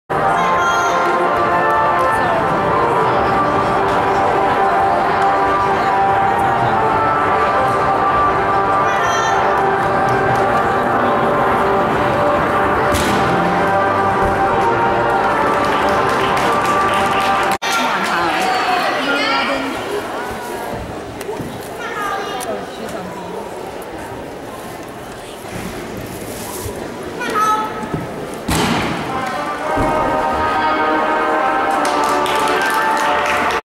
Holly Biggs at Maryland Classic 2013 Vault Event Finals Jan 2013
Scored 9.125 and placed 5th.